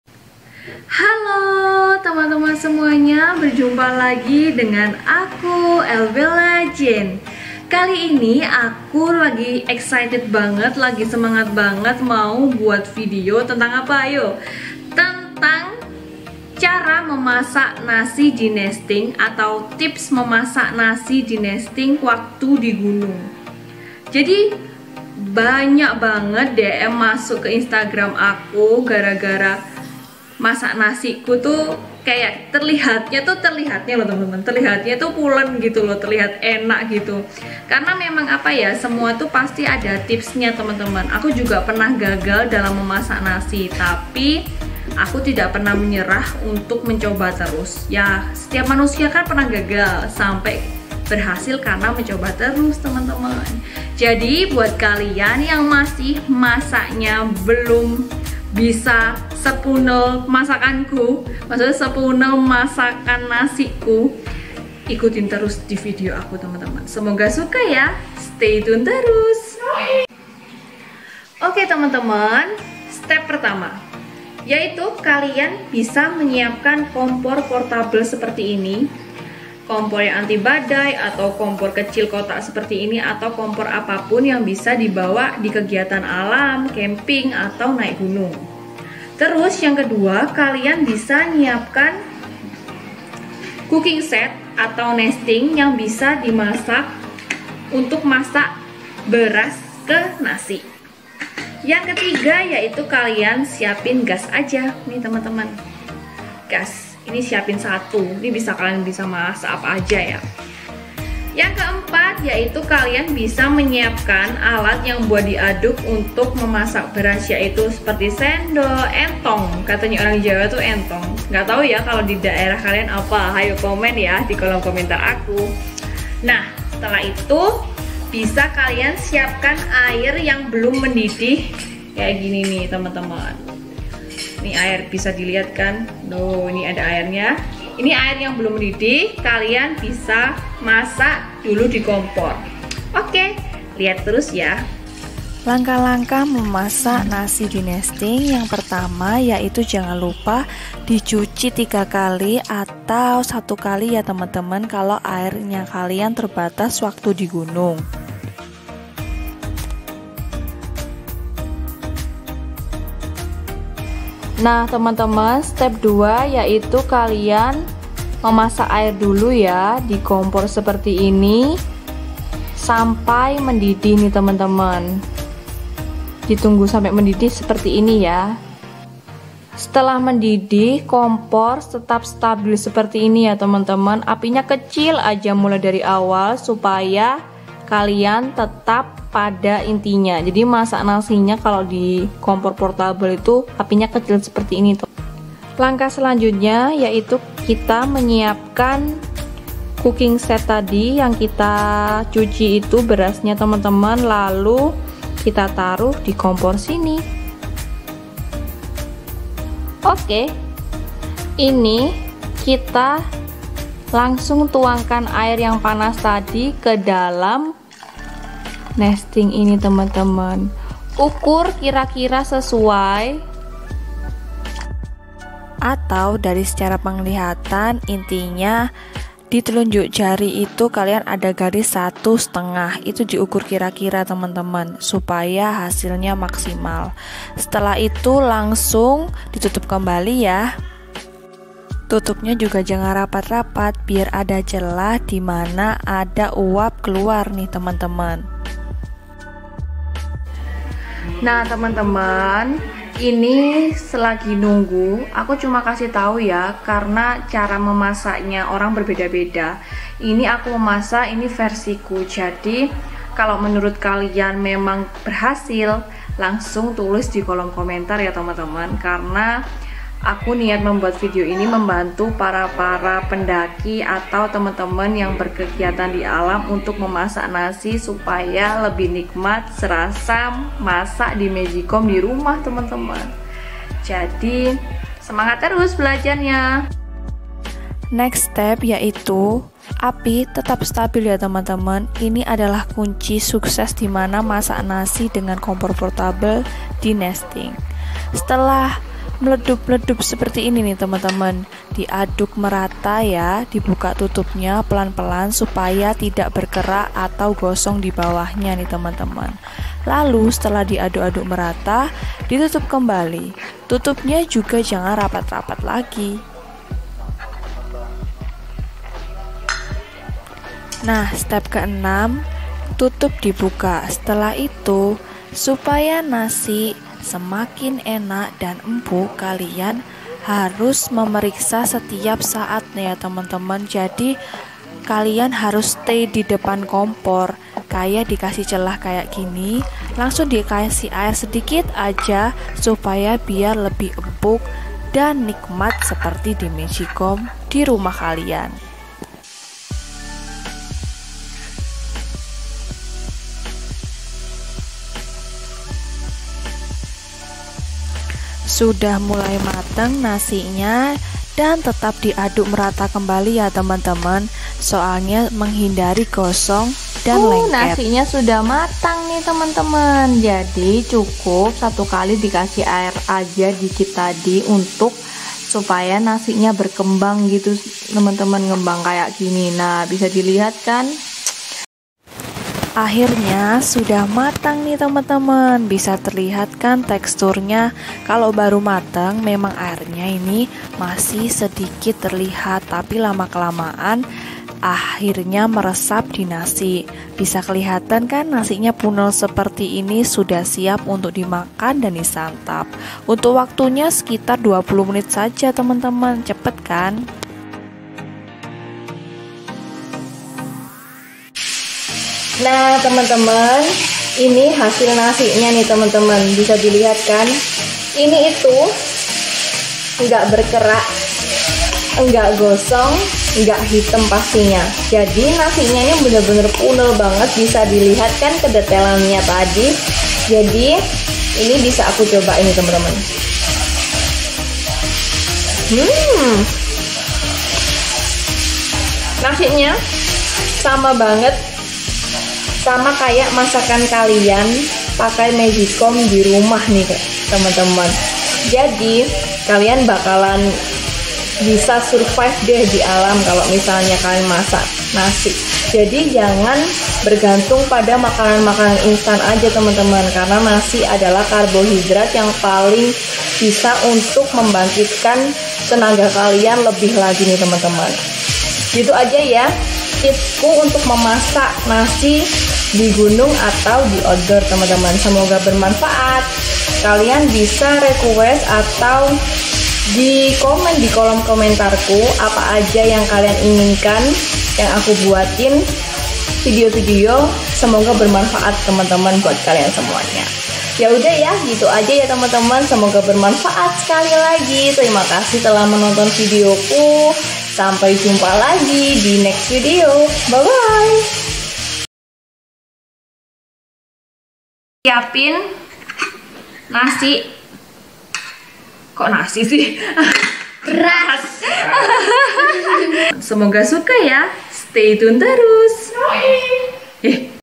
CARA MASAK NASI DI GUNUNG | NESTING + COOKING SET
Hay teman teman semuanya!
Kali ini aku mau kasih tips tutorial masak nasi di gunung pakai nesting atau cooking set versi bella yah.

Semoga bermanfaat buat teman teman yang mau naik gunung. Aamiin
Stay tune yah sampai selesai di videoku!